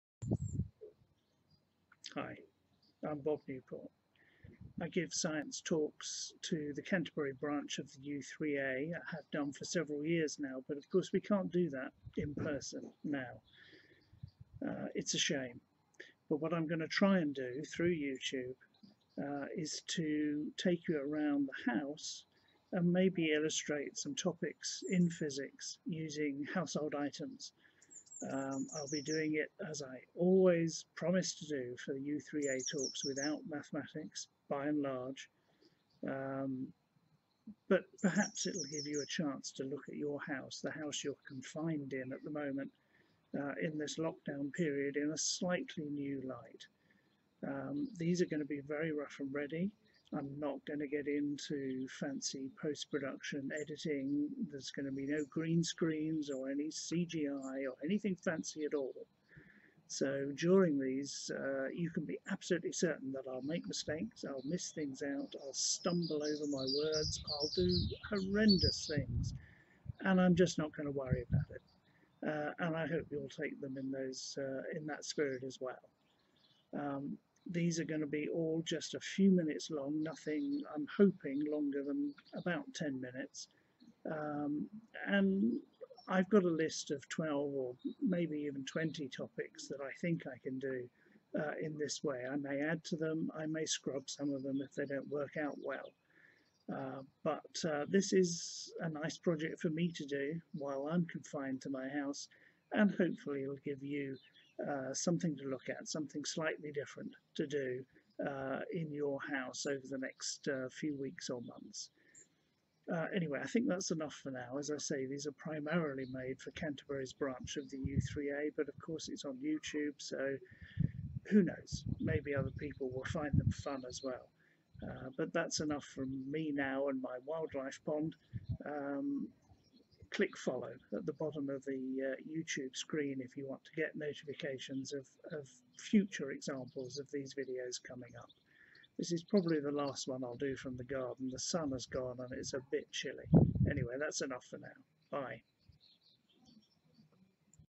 Physics in the House: Introduction
An introduction to a series of short talks in which I'll try, with the minimum of mathematics, to show how everyday items and processes in the home point the way to important aspects of physics. Each video  in the series will be approximately 20 minutes long, give or take.*

This project was inspired by and is primarily aimed at the lovely members of Canterbury's U3A branch: we might not be able to gather together to learn during the current pandemic, but that shouldn't stop us trying. I will attempt to augment these (extremely amateur) videos with posts on my blog but there is likely to be a delay as such things take a little time. I'll certainly maintain a rolling 'Contents Page' with links to the various videos in the 'Physics in the House' series

* Indeed, I've already had feedback that the few posted thus far - up to #7 - are too long. I'll try to do something about that in subsequent videos, but I'm not going to re-make any already posted. There are also a few mistakes - nothing too horrendous thus far ...